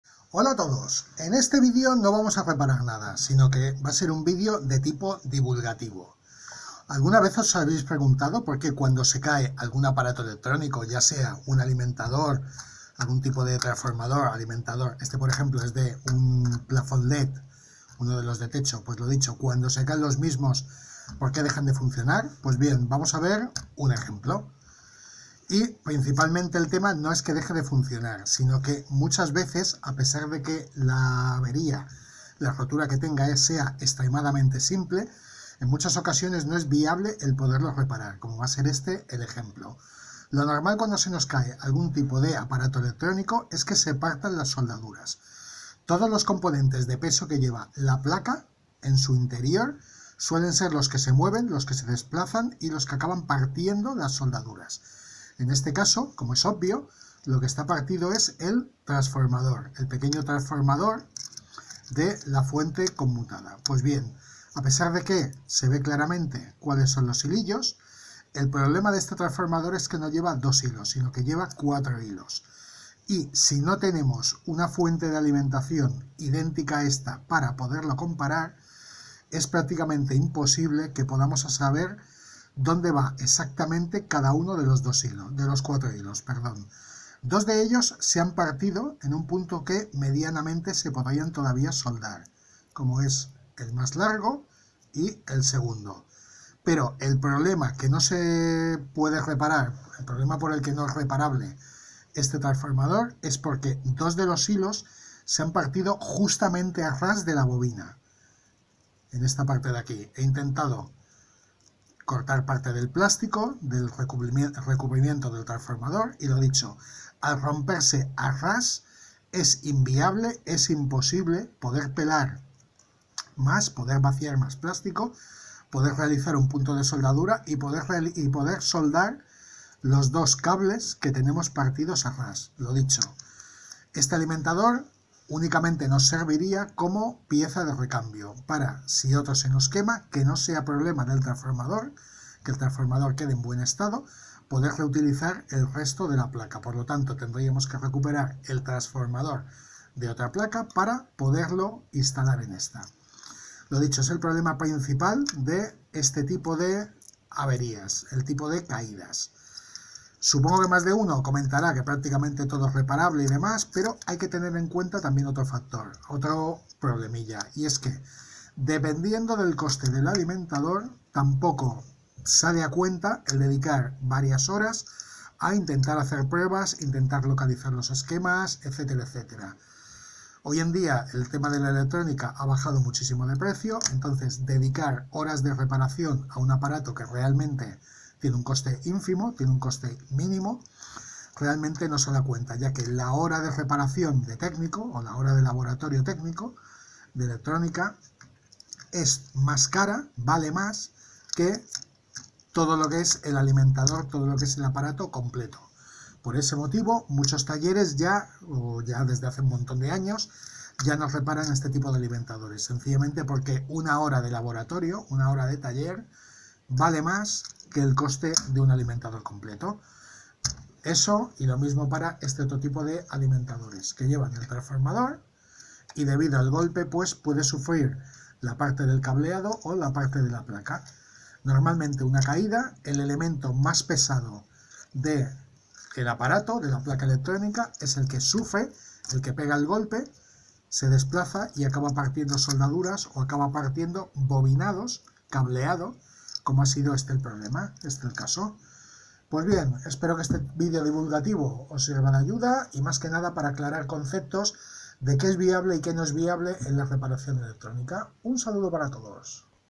ELECTRONICA BASICA: QUE SE ROMPE EN UNA CAIDA DE UN APARATO O ALIMENTADOR
¿QUE COMPONENTE SE AVERÍA EN UNA PLACA ELECTRÓNICA ANTE UNA CAÍDA?  Generalmente el componente más pesado de la placa, que suele ser el transformador, rompe sus cableados o las soldaduras que los sujetan, en ocasiones es reparable rehaciendo soldaduras, pero en otros casos como el del video no es viable, también hay que tener en cuenta el coste  de las horas que tardaremos haciendo pruebas e intentando reparar.

En electrónica y menos cuando es una actividad laboral la reparación profesional, no debemos empecinarnos en reparar algo cuando no es viable o no vamos a poder cobrar TODAS LAS HORAS QUE DEDIQUEMOS A ELLO,  en alguna ocasión por X motivo se puede hacer pero no  de forma habitual, sino la empresa está abocada al cierre.